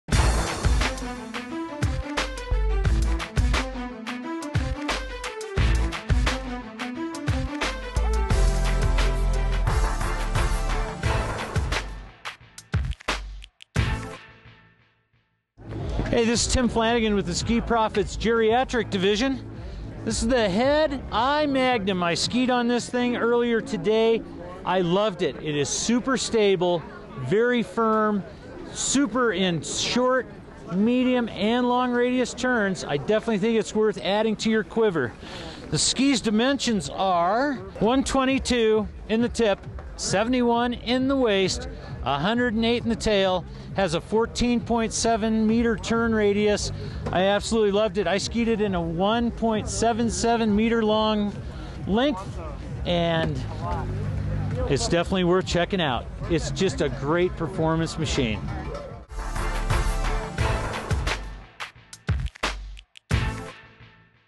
SkiGearTV Ski Test of the 2012 Head "i.Magnum" By Tim Flanagan
Tim Flanagan with the Ski Prophet's "Geriatric Division" scares himself on the 2012 Head "i.Magnum" in this Ski Test for the SkiGearTV Buyer's Guide.